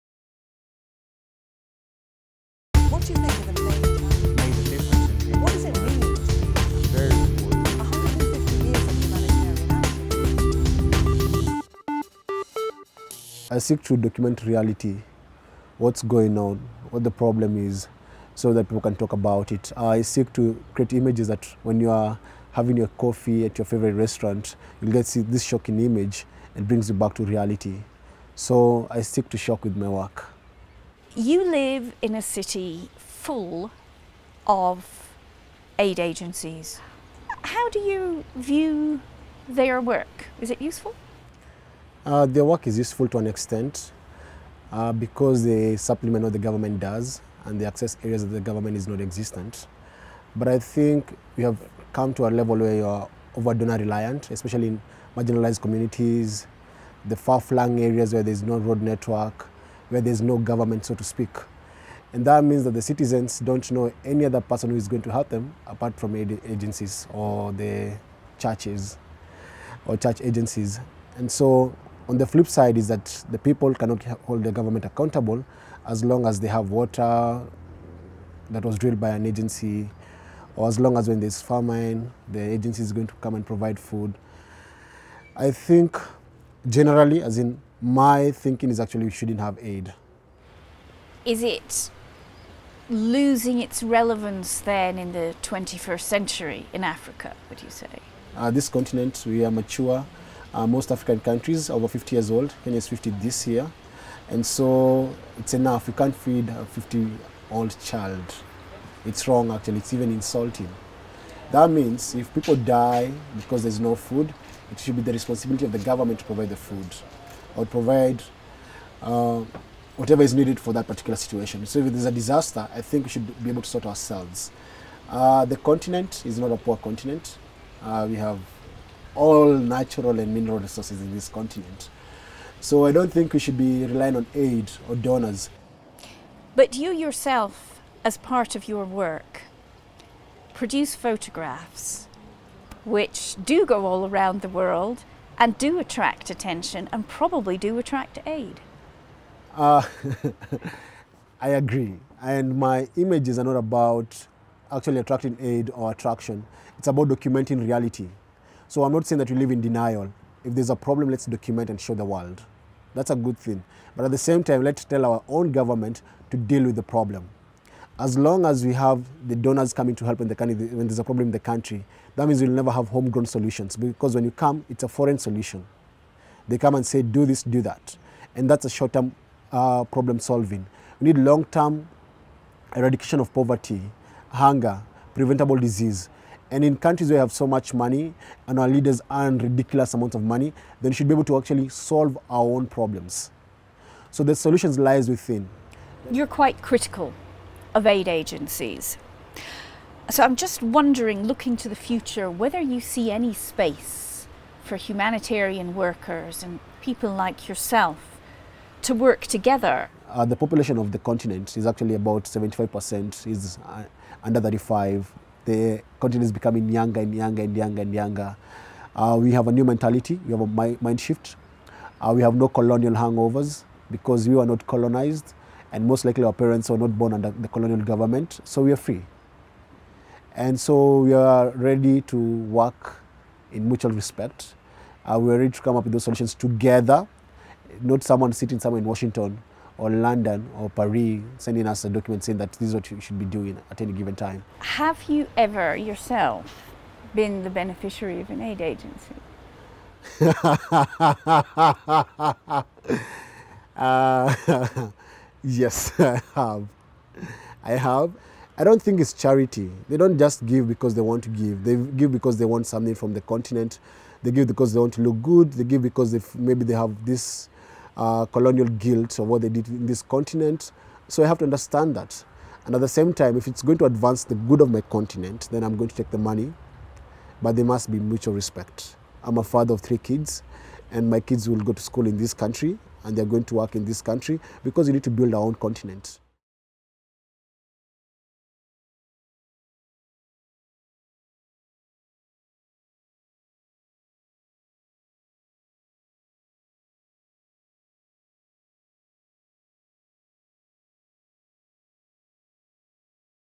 Kenya: We don't need aid
In conversation with photographer Boniface Mwangi. He talks about his work and explains why he believes Kenyans should not be on the receiving end of international aid.

"In conversation with" is a video series featuring diverse views on how to meet the humanitarian challenges of the 21st century. The views expressed do not necessarily represent those of the ICRC.

AV098A